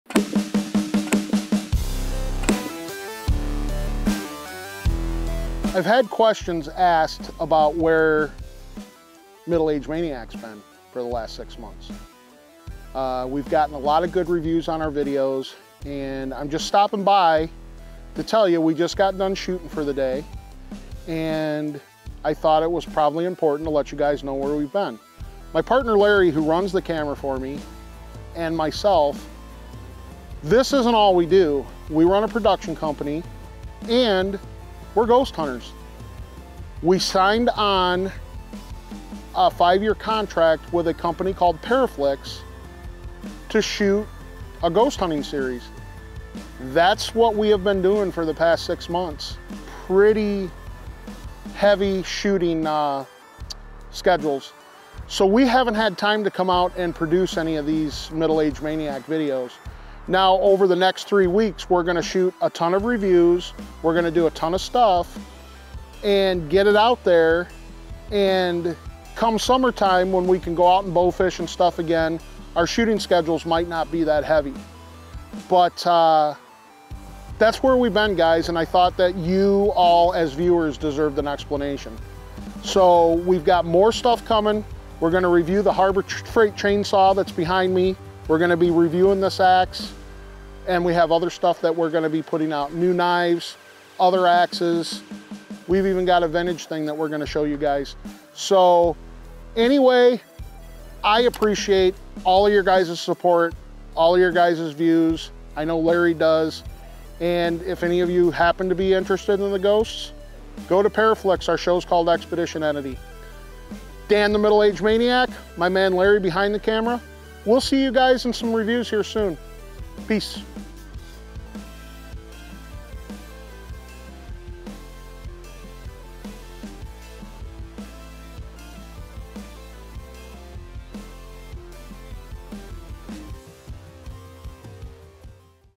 We're Back At It With Gear Reviews!
We had to take a little break from doing reviews and this video will help explain that. Be ready to see some more gear reviews soon!

Always remember to be cautious with your gear to prevent injuries!